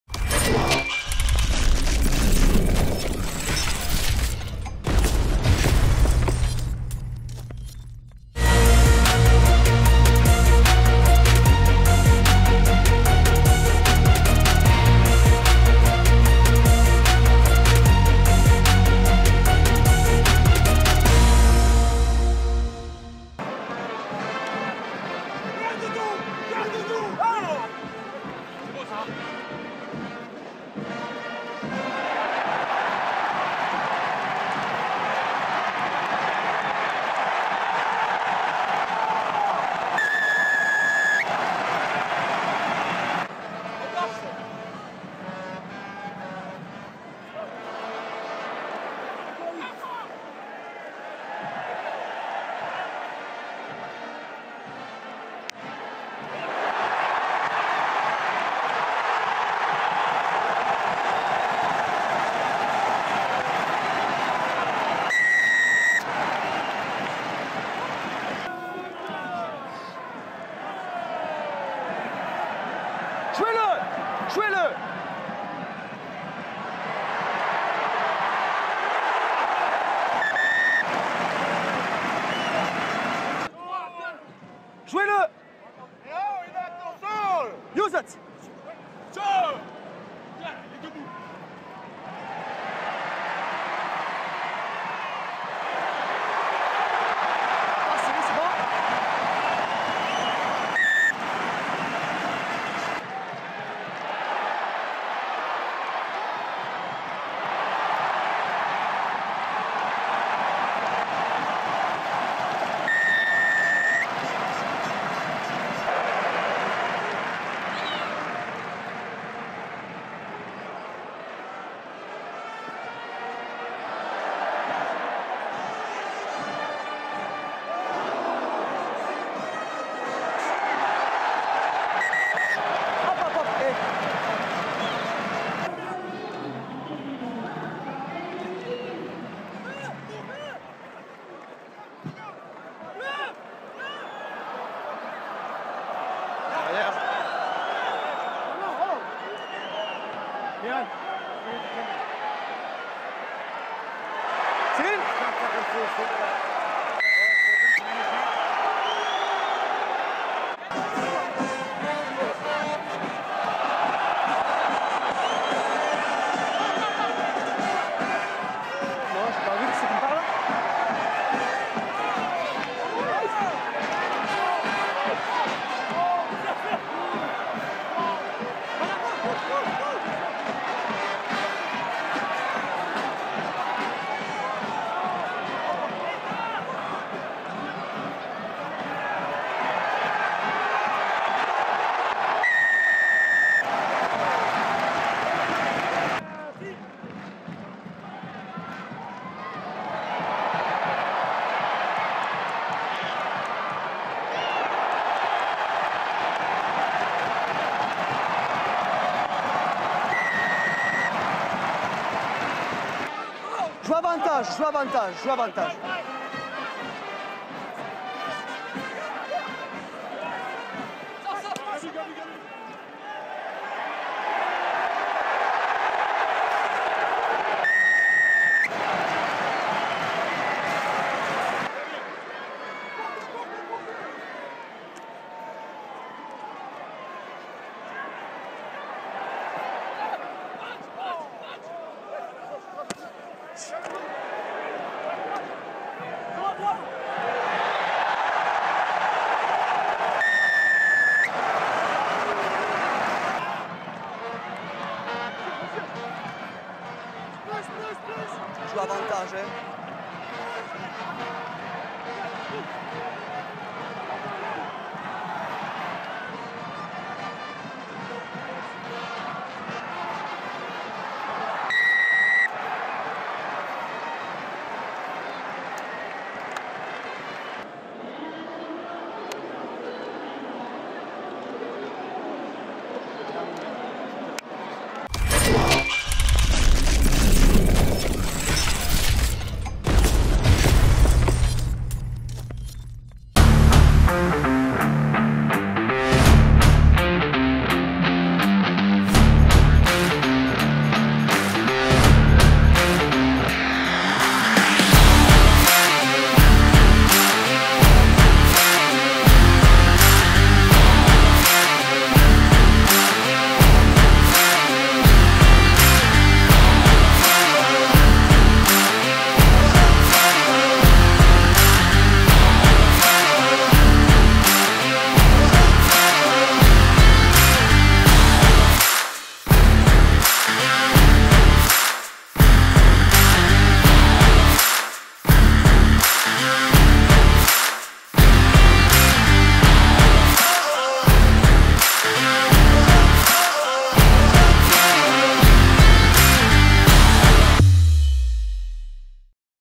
MHR - Stade (Top 14 - J5)
Revivez les temps forts de la rencontre des Rouge et Noir face au MHR lors de la 5ème journée de Top 14.